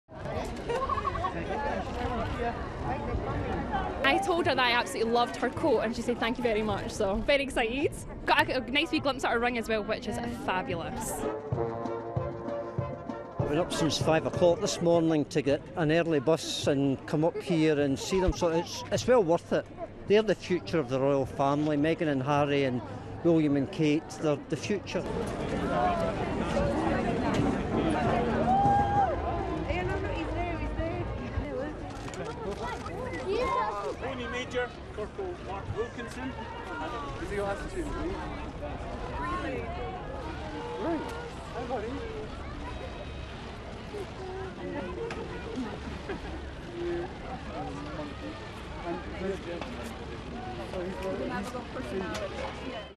Prince Harry and tartan-clad Meghan Markle excite Edinburgh
Prince Harry and his American fiancee Meghan Markle, who was dressed in a Scottish tartan coat, were welcomed enthusiastically in Edinburgh on Tuesday on an official joint visit to Scotland ahead of their marriage in May.